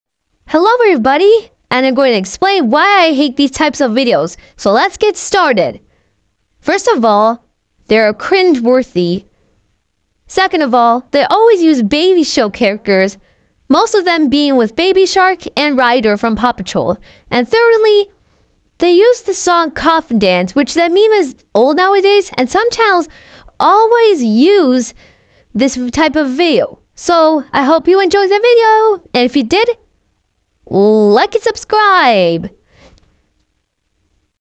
Why i HATE these types of videos (the tiles hop battle)rant
if you like these videos, i can handle your opinion.